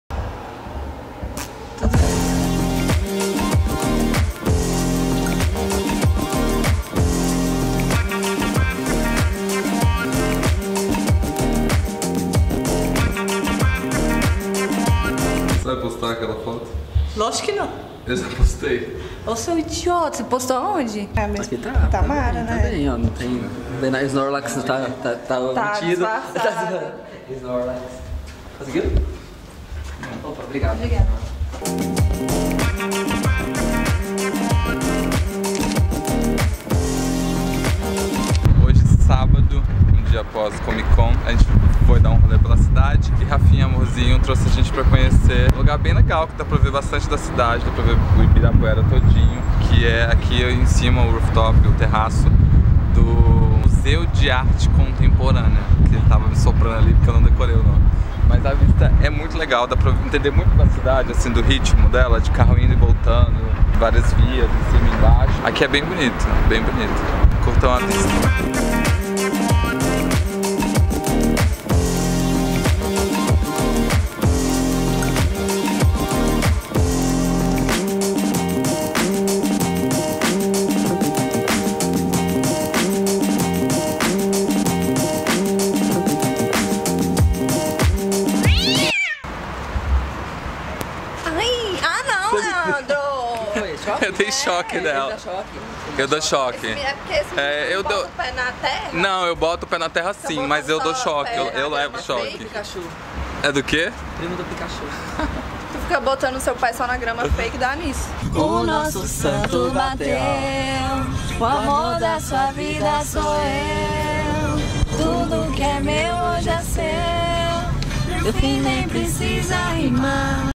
MELHOR VISTA DE SÃO PAULO | VLOG LE MORAIS
Após Comic Con, Amorzinho levou eu e Priscilla pra ver um dos cantos da cidade do alto.

| Snap: lemorais